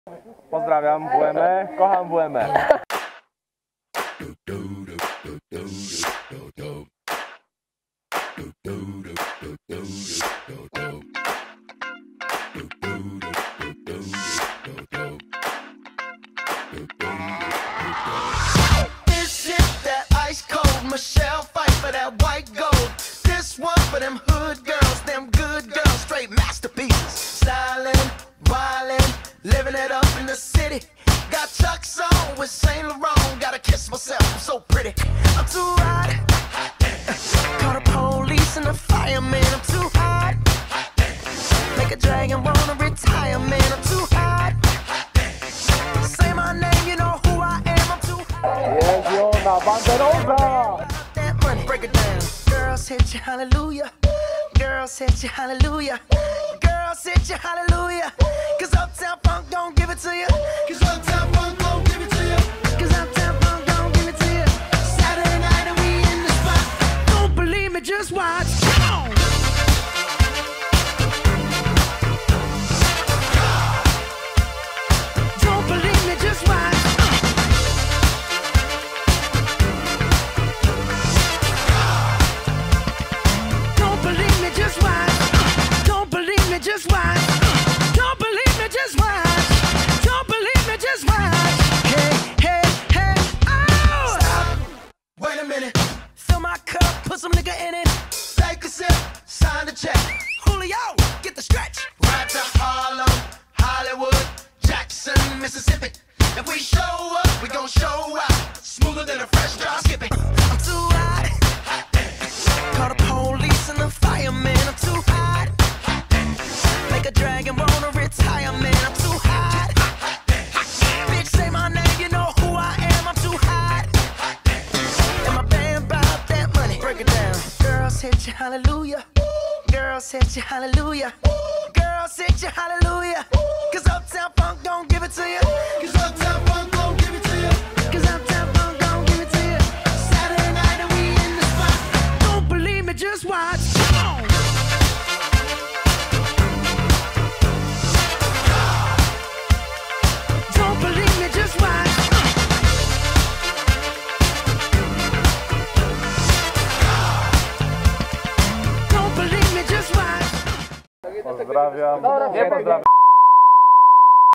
PWr WME - XII RAJD ENERGETYKA - 2016 Banderoza Głuchołazy
XXI Rajd Energetyka -  Rajd Wydziau Mechaniczno-Energetycznego (W-9) Politechniki Wrocławskiej, Banderoza - Głuchołazy 2016